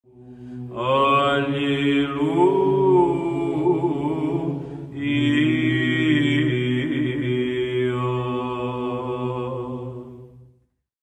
Grace at First Conceals its Presence in the Baptized
St. Diadochos of Photiki ☦️

Want to learn more? Join our Q&A at orthodoxethos.com, or sign up for our mailing list on uncutmountainpress.com! 📩